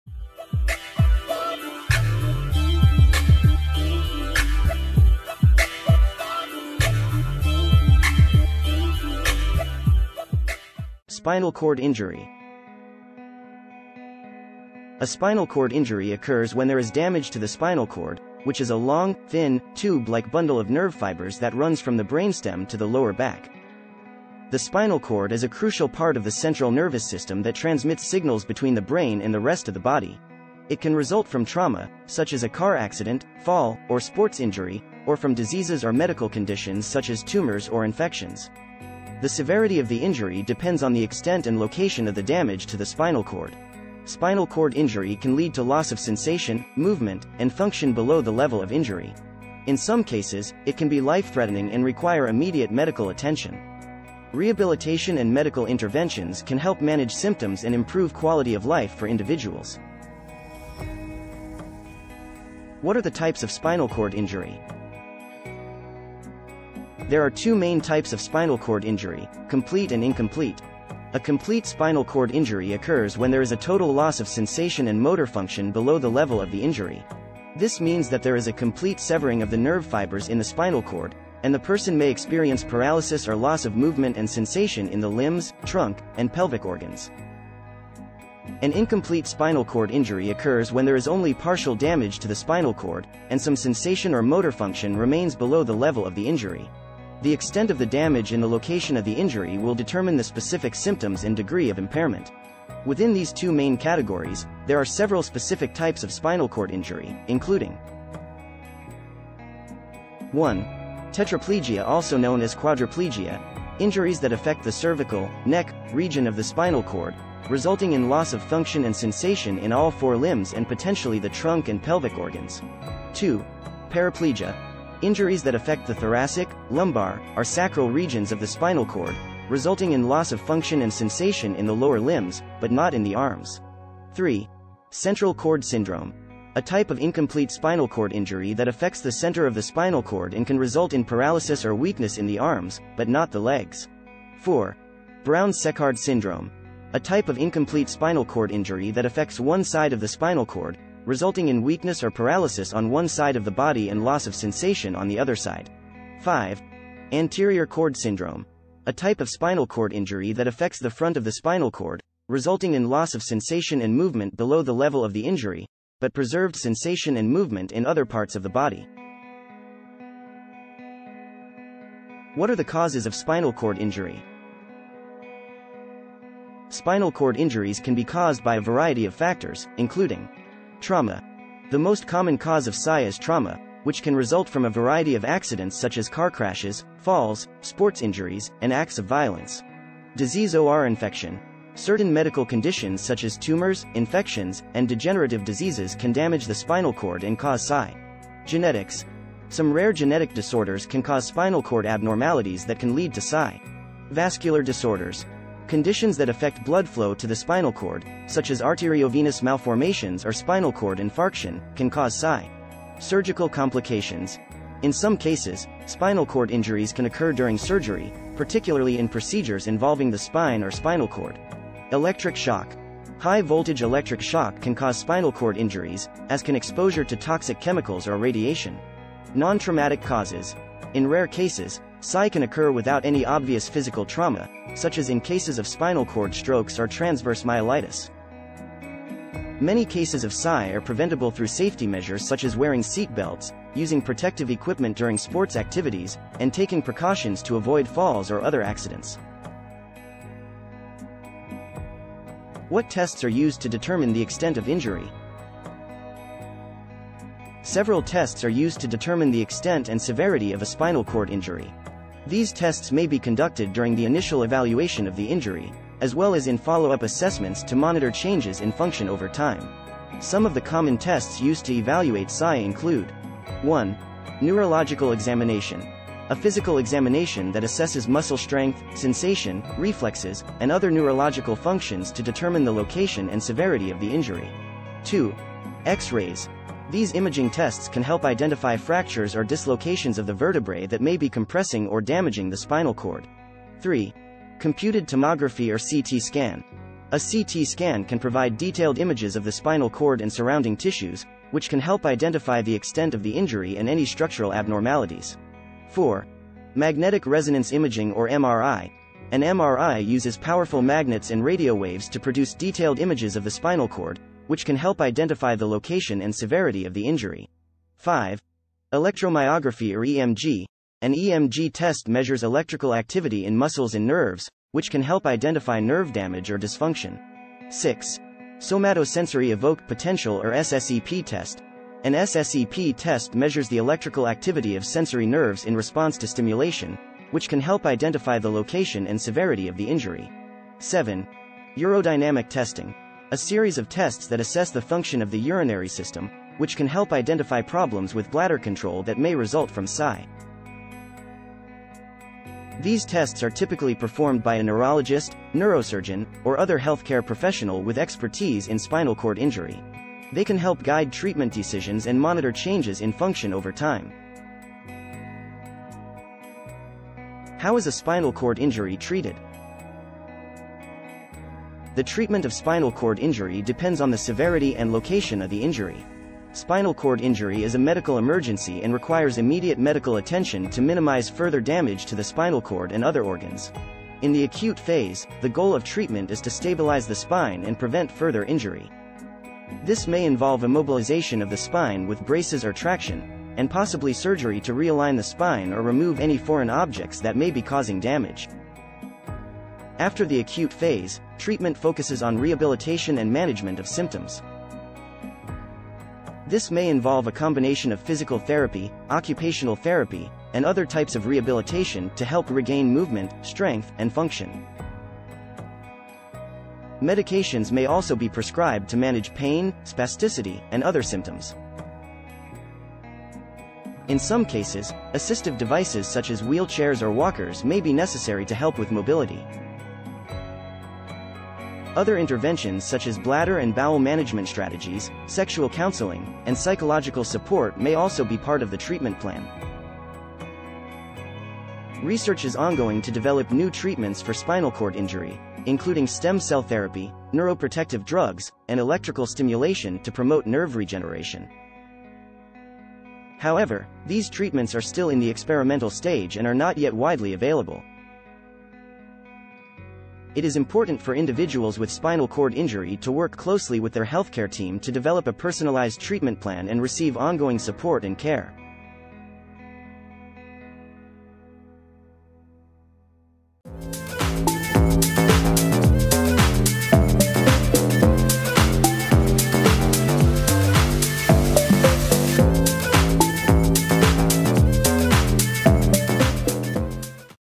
HOW IS A SPINAL CORD INJURY TREATED? | WHAT TESTS ARE USED TO DETERMINE THE EXTENT OF INJURY?
A spinal cord injury or SCI occurs when there is damage to the spinal cord, which is a long, thin, tube-like bundle of nerve fibers that runs from the brainstem to the lower back.

The spinal cord is a crucial part of the central nervous system that transmits signals between the brain and the rest of the body.

It is important for individuals with SCI to work closely with their healthcare team to develop a personalized treatment plan and receive ongoing support and care.